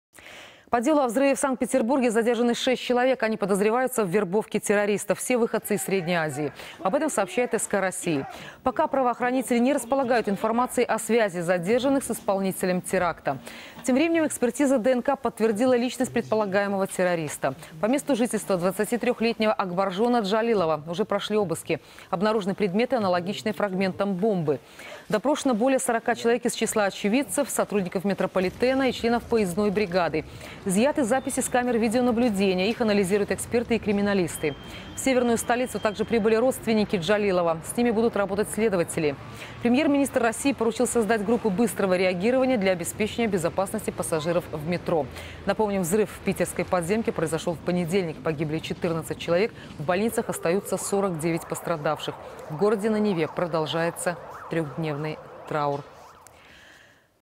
По делу о взрыве в Санкт-Петербурге задержаны 6 человек
Они подозреваются в вербовке террористов. Все выходцы из Средней Азии. Об этом сообщает СК России. Госдума подготовила поправки в Уголовный кодекс - об ужесточении ответственности для вербовщиков террористов вплоть до пожизненного заключения. Пока правоохранители не располагают информацией о связи задержанных с исполнителем теракта. Премьер-министр России поручил создать группы быстрого реагирования для обеспечения безопасности пассажиров в метро. Тем временем по месту жительства 23-летнего Акбаржона Джалилова уже прошли обыски. Допрошено также более 40 человек из числа очевидцев, сотрудников метрополитена и членов поездной бригады. Изъяты записи с камер видеонаблюдения - их анализируют эксперты и криминалисты. В Северную столицу также прибыли родственники Джалилова - с ними будут работать следователи. Напомним, взрыв в питерской подземке произошел в понедельник. Погибли 14 человек, в больницах остаются 49 пострадавших. В городе на Неве продолжается трехдневный траур.